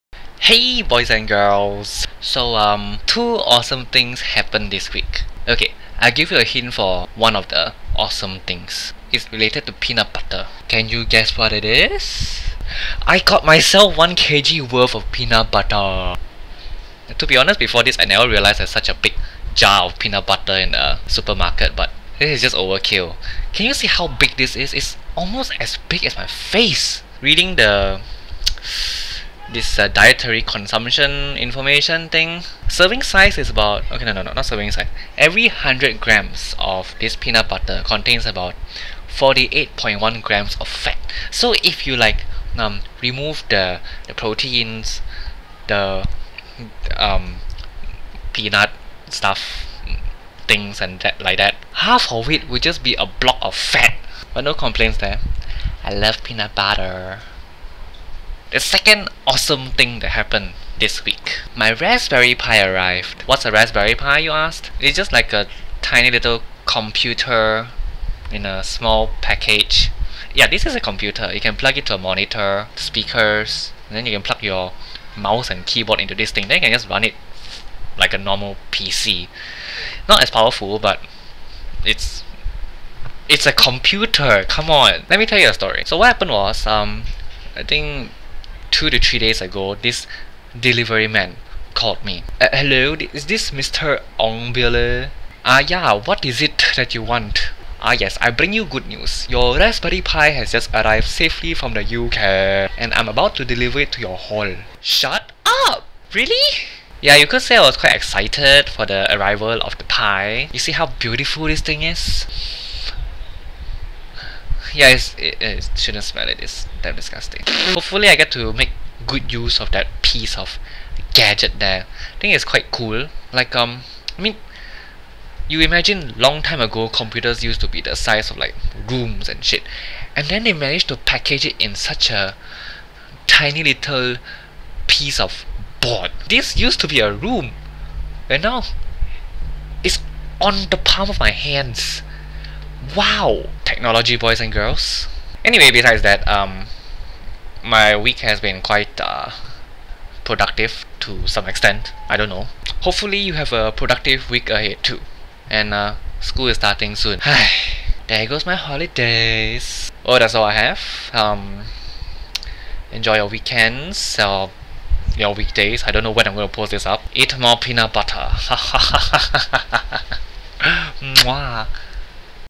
More Peanut Butter... and it's Crunchy!
You know very well my love for peanut butter. Went to the supermarket today and managed to grab myself one big jar of peanut butter. I was... over the moon! If you had been there next to me when I bought it, you would have seen me jumping like a happy little kid who convinced his mum to buy him sweets. Yea...